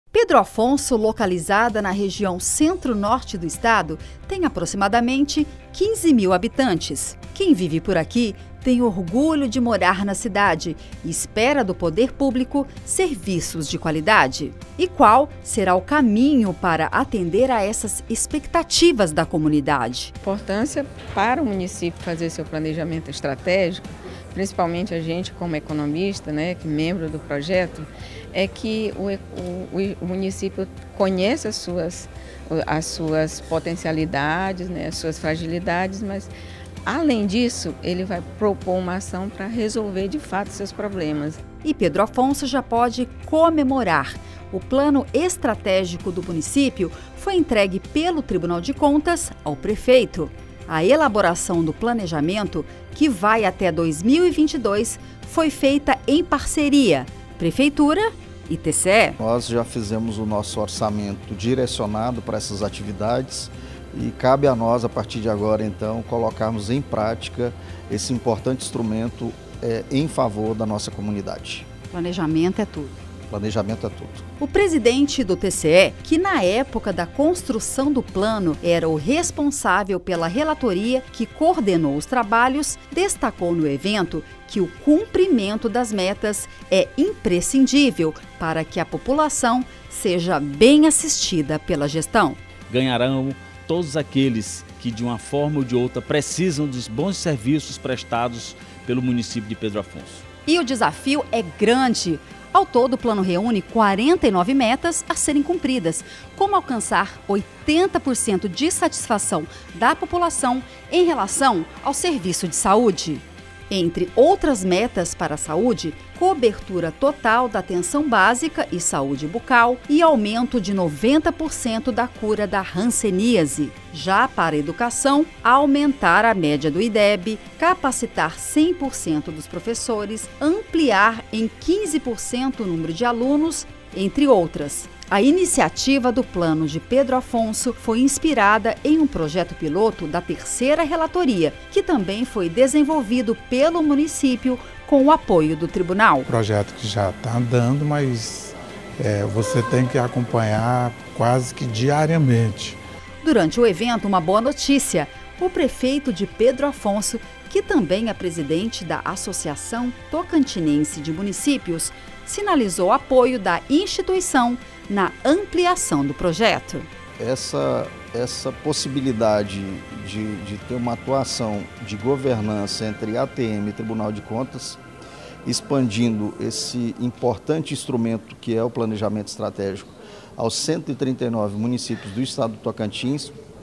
Plano Estratégico do município de Pedro Afonso
Entregue Plano Estratégico de Pedro Afonso, 2018/2022,  desenvolvido pela prefeitura, em parceria com TCE/TO e Corecon.